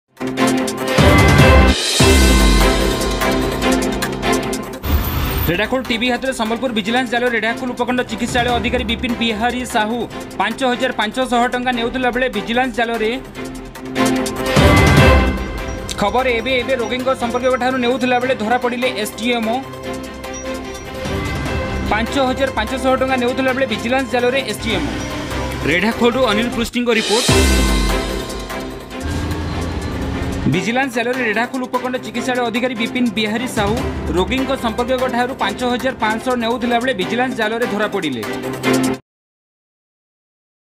REDHAKHOL KHABAR
ଅସ୍ତ୍ରୋପଚାର ପାଇଁ ୫୫୦୦ଟଙ୍କା ନେଉଥିବା ବେଳେ ଭିଜିଲାନ୍ସ ଜାଲରେ ରେଢ଼ାଖୋଲ ଉପଖଣ୍ଡ ଚିକିତ୍ସା ଅଧିକାରୀ